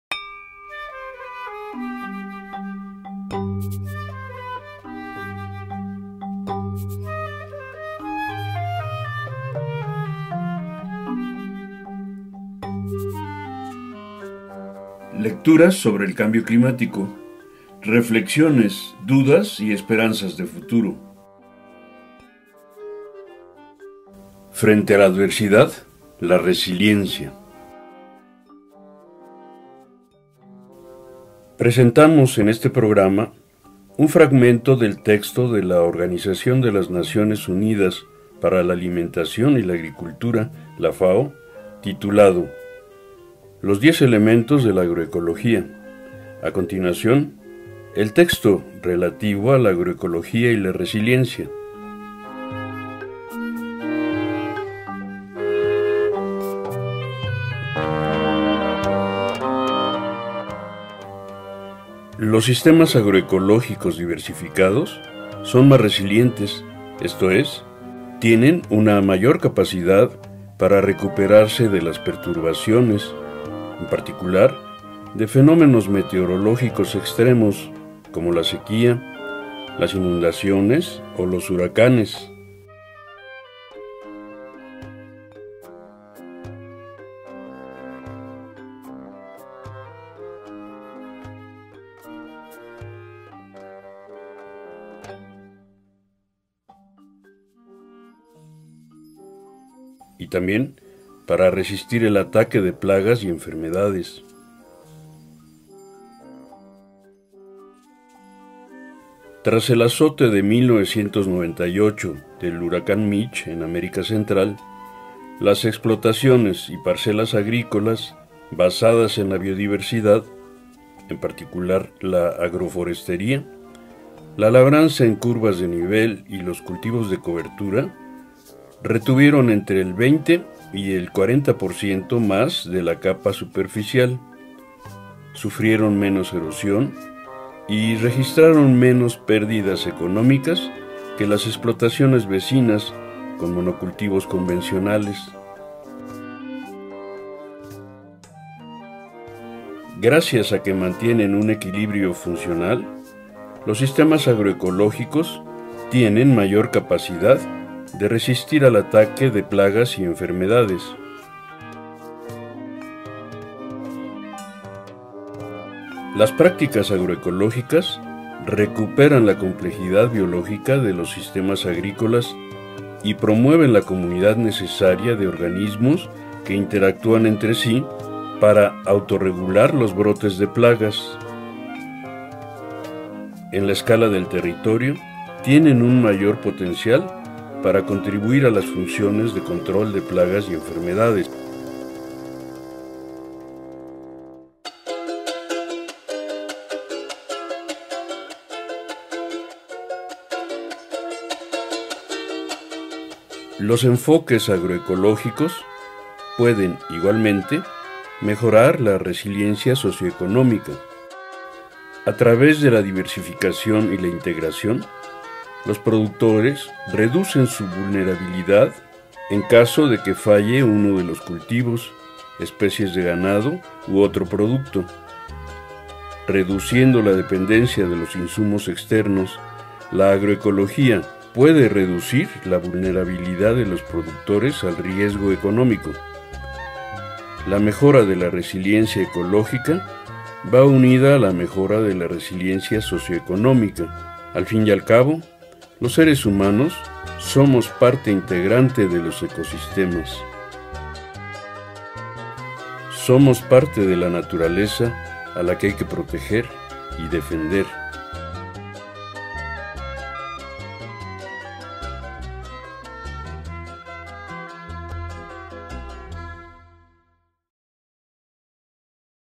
25. Los 10 elementos de la agroecología FAO / La resiliencia socioambiental frente al CC
3. La resiliencia socioambiental frente al cambio climático

25. Los 10 elementos de la agroecología FAO
(Internet, FAO)

1. Lecturas sobre el cambio climático

2. Testimonios campesinos sobre los cambios climáticos

Créditos:

Producción
GEAVIDEO

Catherine Marielle

Mexico-Tenochtitlan 2019.